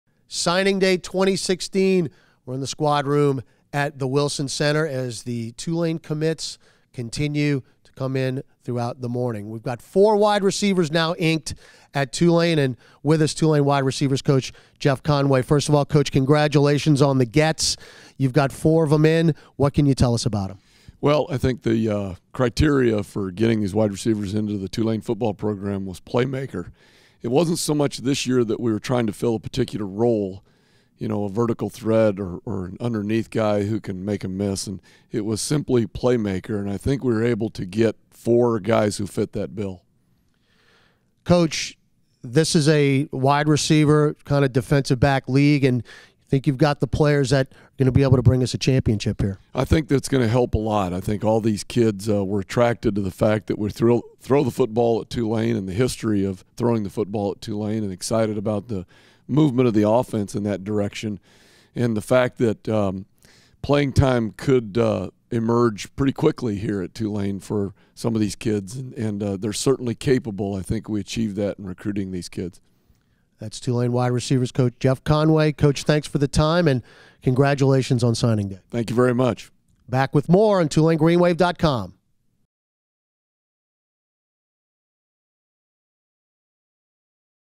Tulane Football National Signing Day Interview With Coach Jeff Conway
Tulane Football wide receivers coach Jeff Conway talks about the receivers that the Green Wave picked up on National Signing Day.